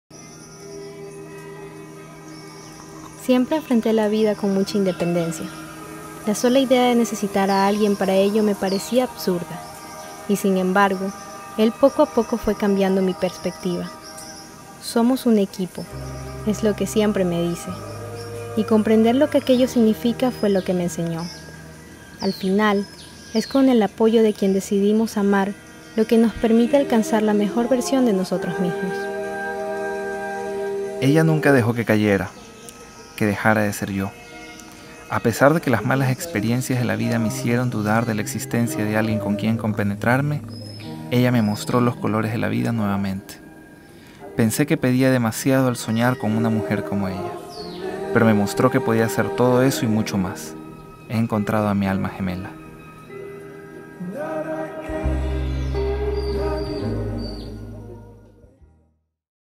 Film Preboda - Hector & Amy
Hector & Amy son sin duda alguna el uno para el otro, nos encanto plasmar sus ideas y contar por medio de un video un poco de lo que sienten y el por que son "almas gemelas".
Felicidades en esta nueva etapa de sus vidas.
¡Que vivan los novios!